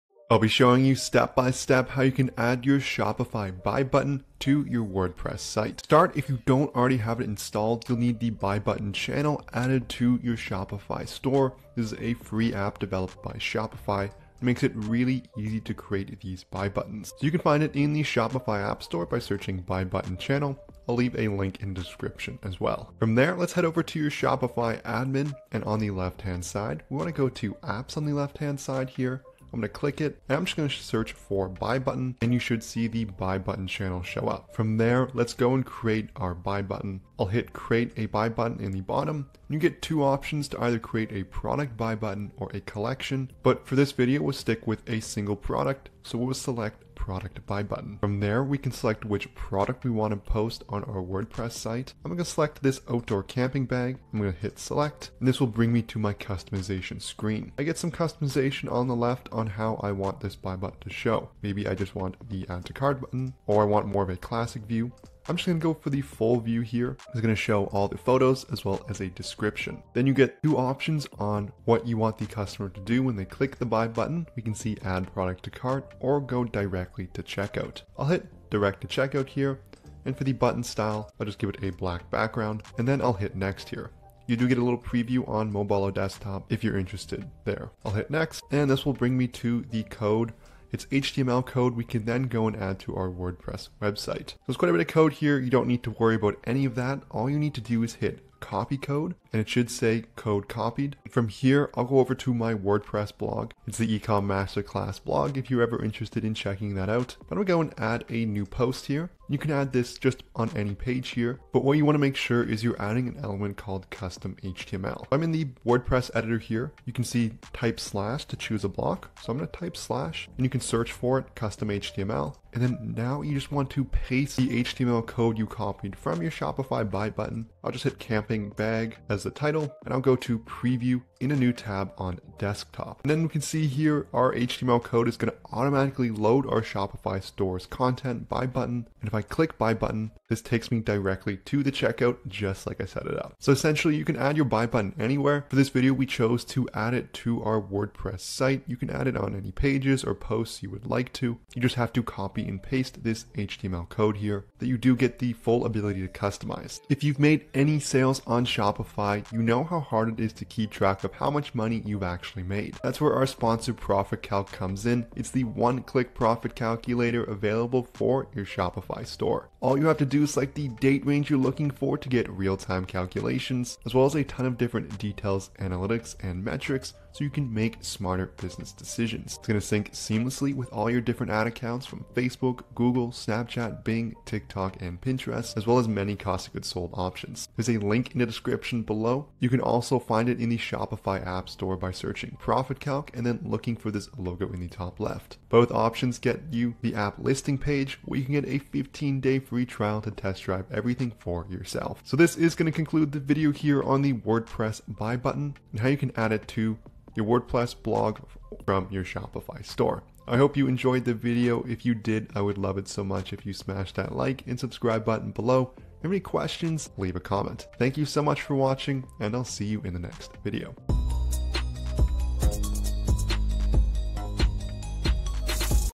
Shopify Buy Button on Wordpress
COPYWRITING ROBOT
🛒 Learn How to Add Shopify Buy Button on WordPress! 💻 Create a Seamless Shopping Experience on Your Website! 📈

Are you a WordPress user who wants to integrate the power of Shopify into your website? Look no further! In this video, we'll show you step-by-step how to add the Shopify Buy Button to your WordPress site, allowing you to sell products effortlessly and boost your online business.

🌟 What is the Shopify Buy Button? 🌟
The Shopify Buy Button is a versatile and powerful tool that enables you to showcase and sell your Shopify products directly on your WordPress website. Whether you're a blogger, a content creator, or a small business owner, this integration will help you monetize your content and drive sales without redirecting your customers to a separate e-commerce platform.

⚙️ How to Add the Shopify Buy Button on WordPress ⚙️
We'll guide you through the simple process of setting up the Buy Button on your WordPress site. From generating the code in your Shopify admin to customizing the button's appearance and functionality, you'll have a fully functional shopping experience seamlessly integrated into your website.

🎯 Why Should You Use the Shopify Buy Button? 📊
Integrating the Shopify Buy Button into your WordPress site offers numerous benefits. Not only does it save you time and resources by utilizing Shopify's secure checkout process, but it also maintains a consistent user experience for your customers. Plus, with Shopify's robust inventory management, order tracking, and payment gateways, you can focus on growing your business with peace of mind.

🚀 Ready to level up your online sales game? Don't miss out on the opportunity to combine the best of both worlds - WordPress and Shopify! Watch the video now and add the Shopify Buy Button to your WordPress site today.

💡 Got questions or need help during the integration process?